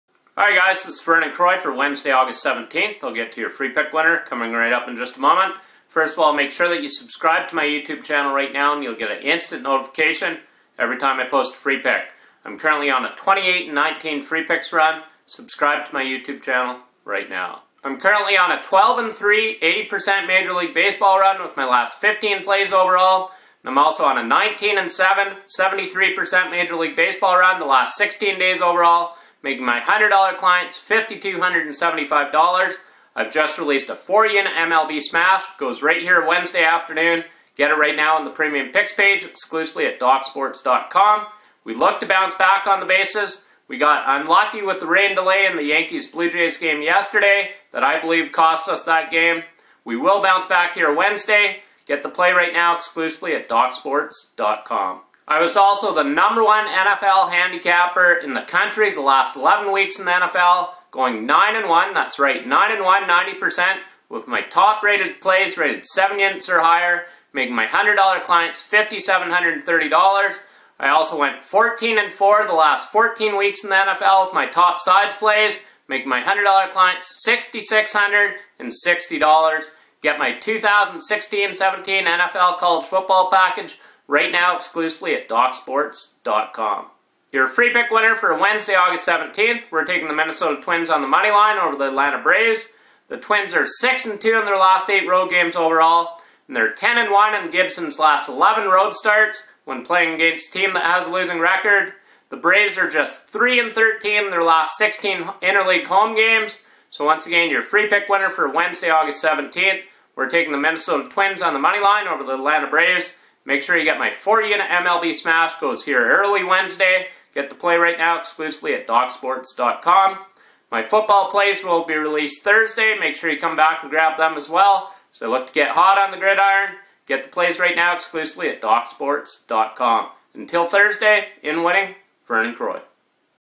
Free MLB Picks - Minnesota Twins vs Atlanta Braves Prediction 08/17/16 7:10PM ET
Todays free MLB pick is from handicapping expert Vernon Croy Your free MLB winner for Wednesday, August 17 2016 is between the Minnesota Twins vs Atlanta Braves. to get more free MLB predictions each day as well as $60 free of Croy's premium member only predictions. Croy is on a huge 161-133 free pick video run!